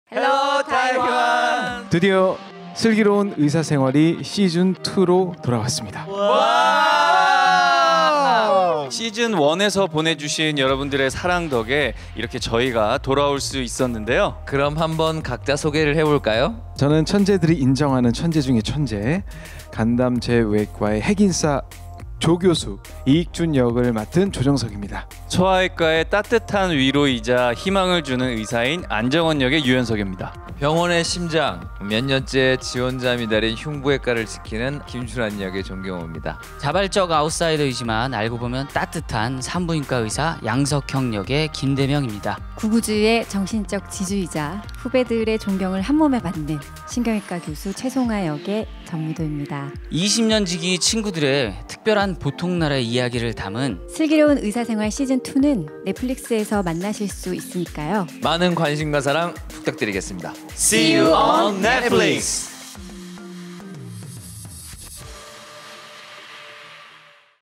《機智醫生生活》回歸啦！主角群跟台灣問候影片公開！｜ELLE Taiwan
第一集播出，首都圈平均收視率爲11.7%，最高收視率爲14.9%，全國平均收視率爲10%，最高收視率爲12.4%。
所有頻道中佔據了第一名。 也是tvN歷代電視劇首播收視率第一名 !